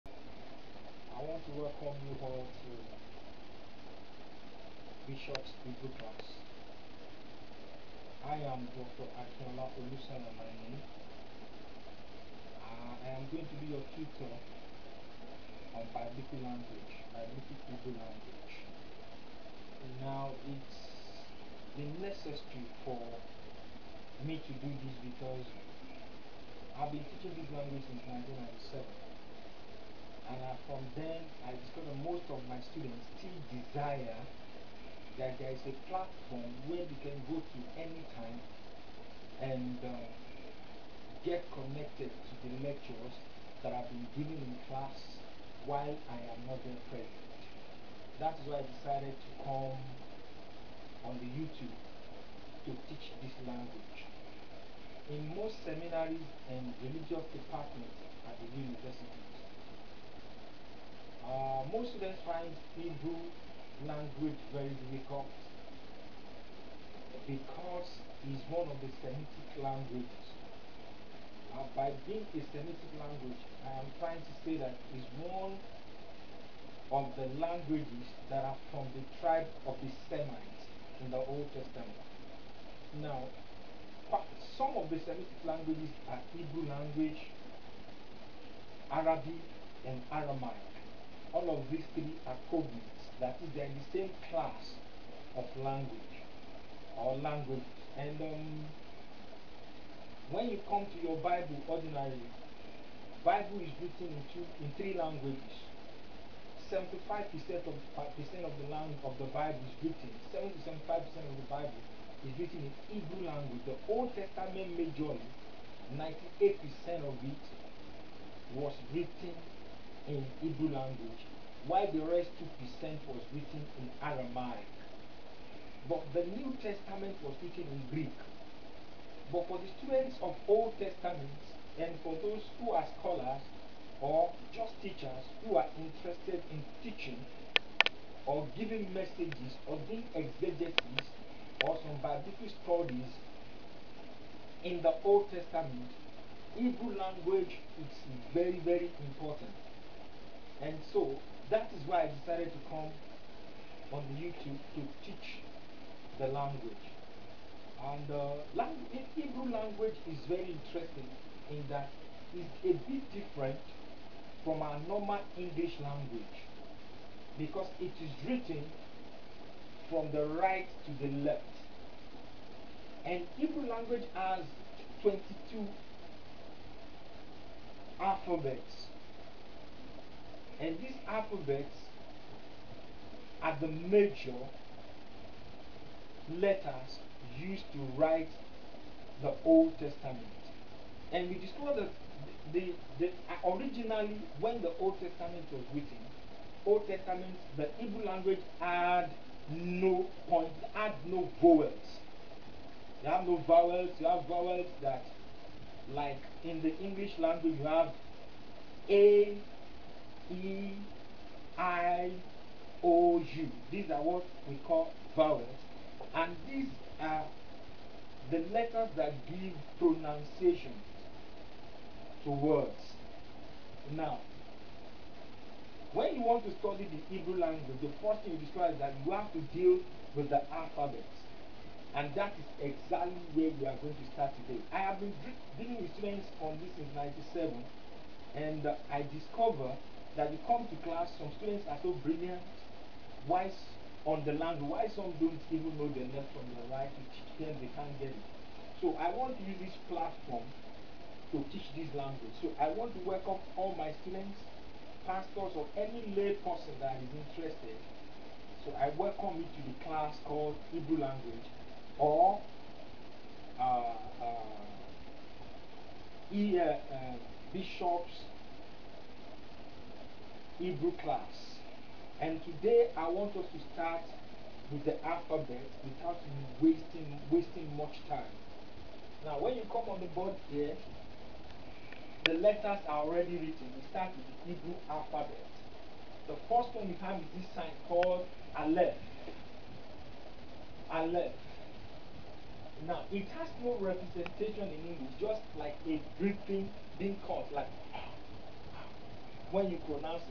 Dr Akinola Bishop Hebrew Class 1
I welcome everyone to this platform called "Bishop Hebrew Class" where I will be teaching basically Biblical Hebrew Language. I am Akinola Olusanya PhD. I am presently the Dean School of Theology of the West Africa Theological Seminary, Lagos, Nigeria.

This platform has been created to meet the needs of my numerous students in various institutions who desire that they have extra class that they can tune into any time to learn again and over what they have been thought. This platform is also open to others who would like to learn Biblical Hebrew to assist them in Biblical studies, Theology, even pastors in preparation of messages.
It is true that most students of Biblical studies and theology find Hebrew language difficult and prefer Greek over Hebrew. So it is the goal of this platform to make learning of Hebrew as easy as possible.
This platform actually started with a Whatsapp page but we have to move up to the youtube.  I invite all my students many others who would find this platform beneficial to come over so that we can continue with our studies from here. So I welcome you all.
Thanks